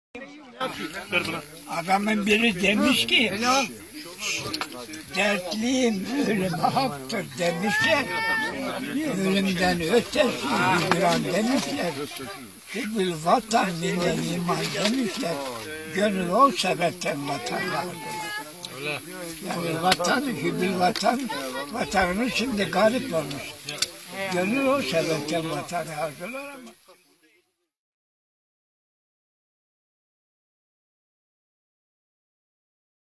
Seyit Rıza'nın torunu Ali Ekber Polat Hakk'a yürüdü
Dersim 38 tanığı ve Seyit Rıza’nın torunu olan Ali Ekber Polat Hakk’a yürüdü.